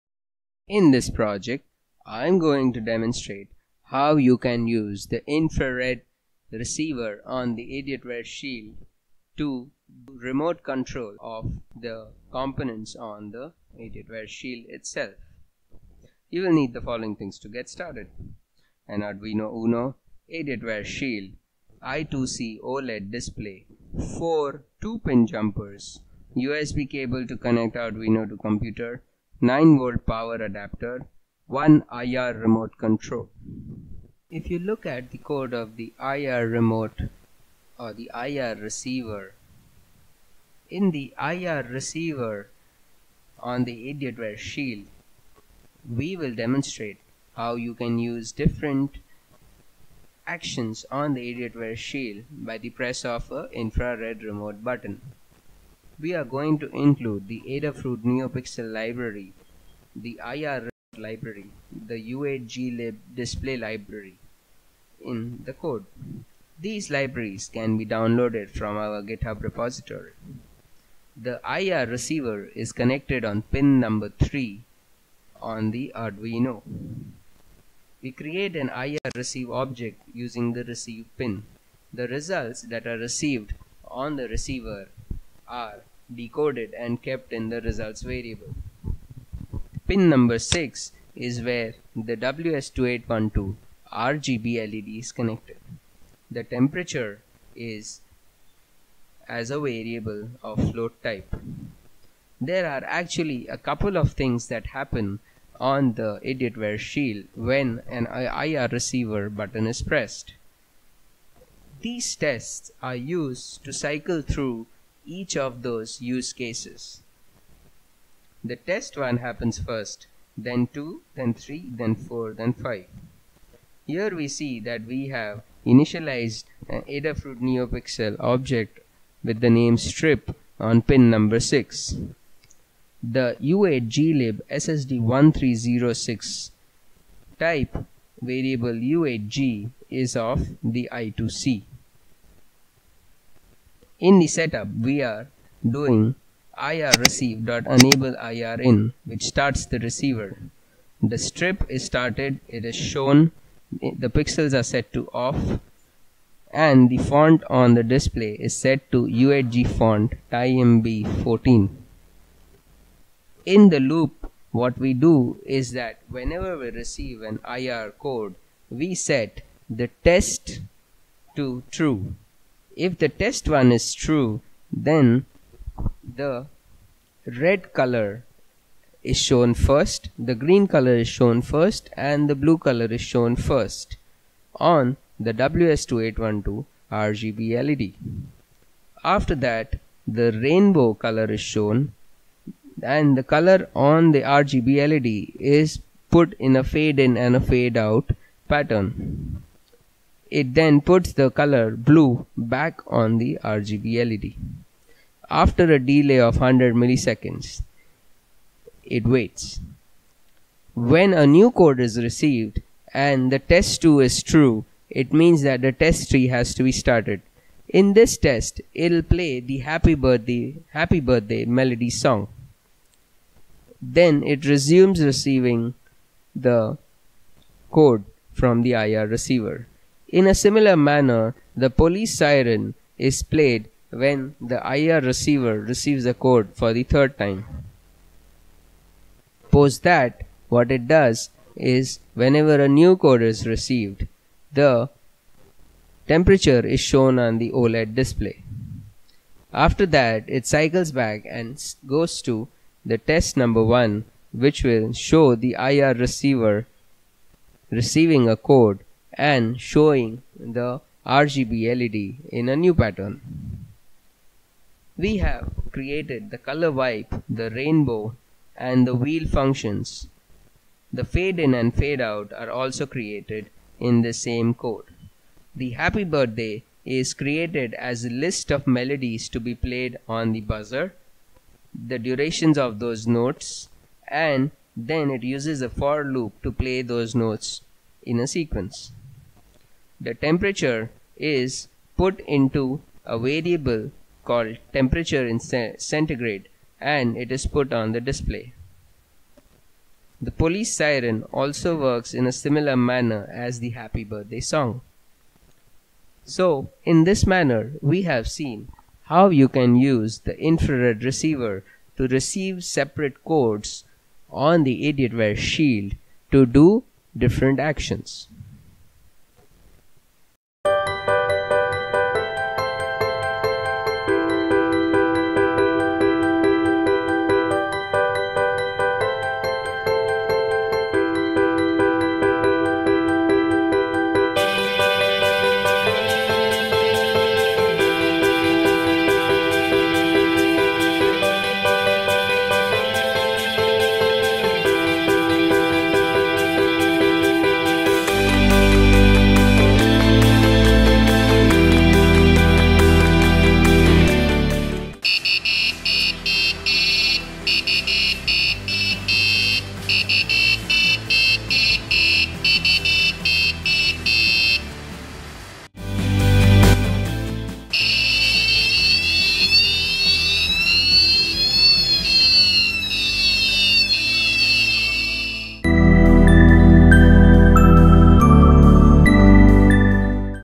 Infrared Reciever Demo and Control Using the Idiotware Shield
In this project I am going to demonstrate how you can control the buzzer and the RGB LED  on the Idiotware shield using an Infrared Remote.